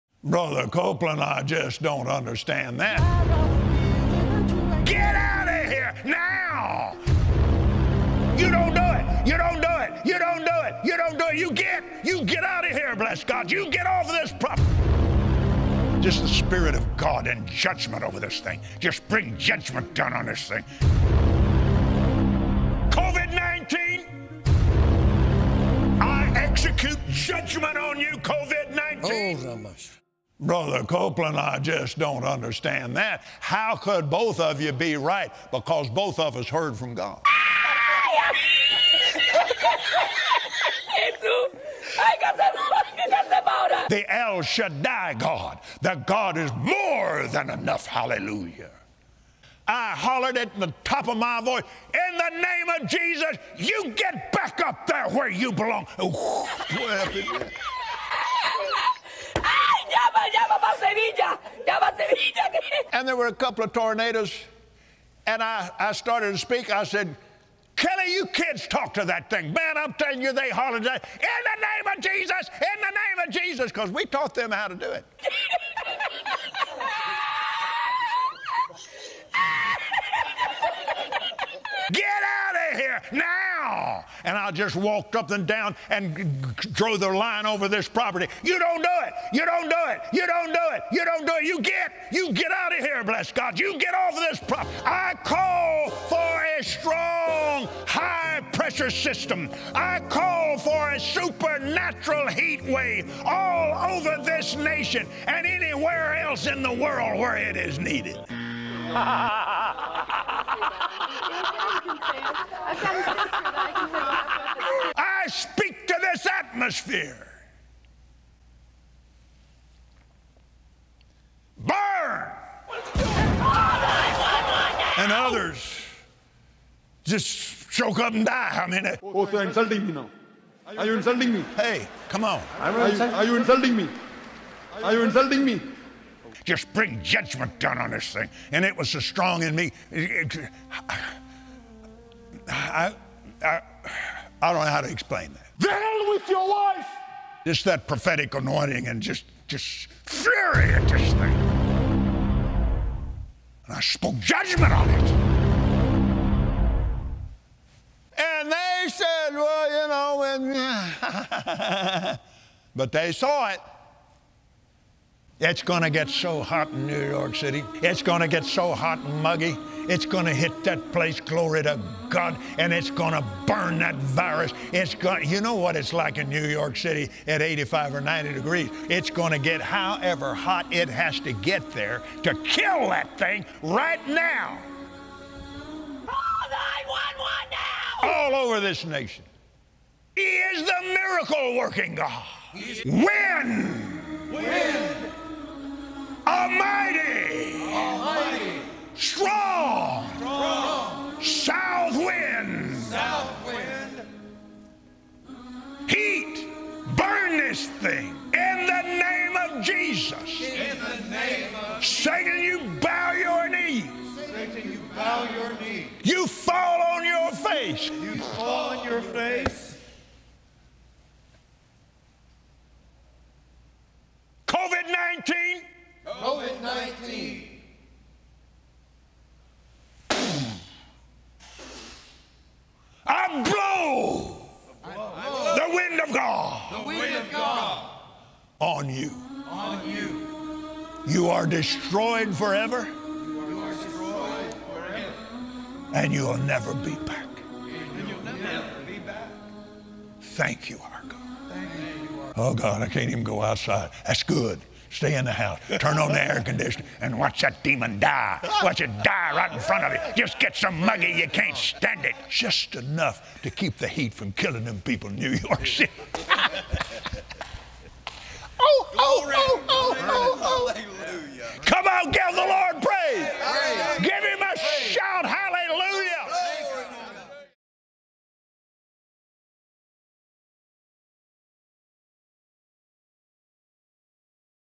A Message To COVID-19 By Pastor Kenneth Copeland (FUNNY)
This multimillionaire Pasotor Kenneth Copeland tried to destroy COVID-19 with ‘the wind of God’.